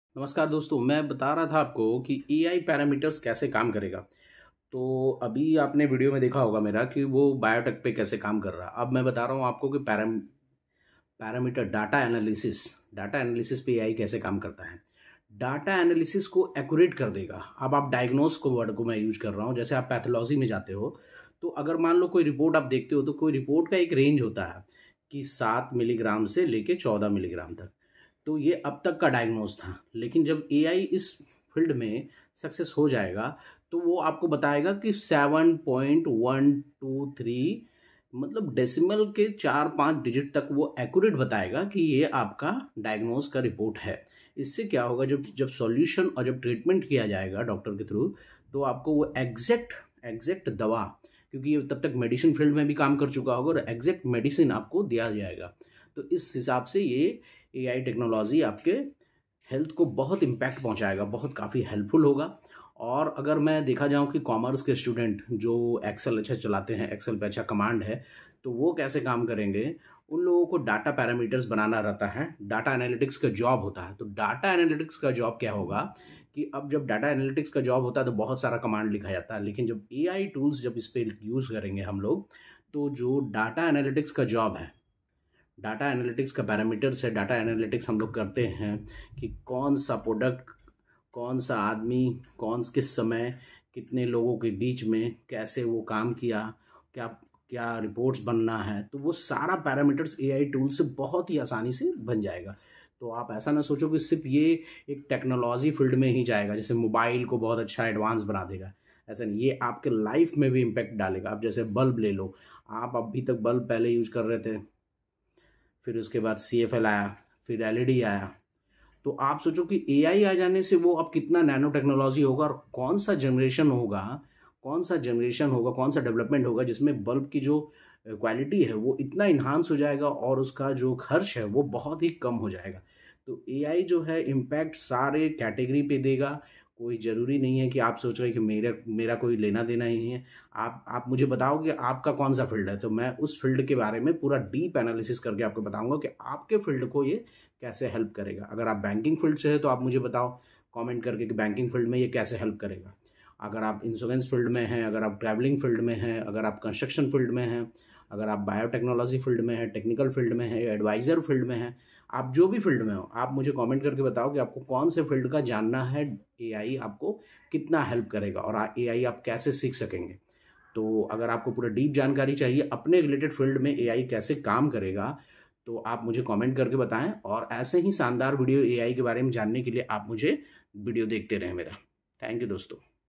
ai help all field openai chatgpt prompt askai sora aap kaun sa field me ai ka help lena chahenge
aap kaun sa field me ai ka help lena chahenge.
Ai हर field को help karega आप kaun sa field me help lena चाहते हो मैं आपका help karunga. Aap mujhe वो field comment box me de.

... आपके अपने language me aapko ai mai बताऊँगा.

Ai REVOLUTION करेगा.
हर field.
ऐसे ही knowledge video or ai को समझने के लिए वीडियो देखे...